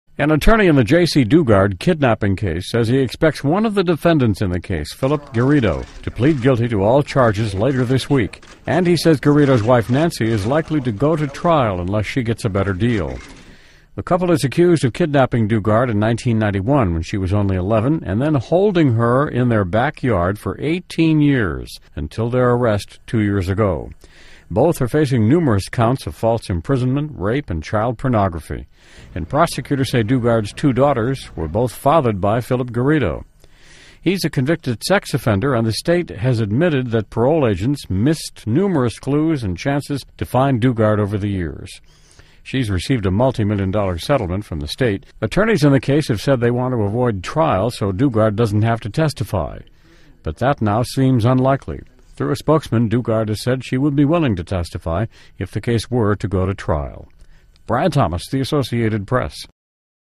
Lawyer Expects Guilty Plea in Calif. Kidnap Case
An attorney in the case of a Northern California girl held captive in a backyard compound for 18 years says he expects the defendant, Phillip Garrido, to plead guilty to all charges later this week.  (April 4)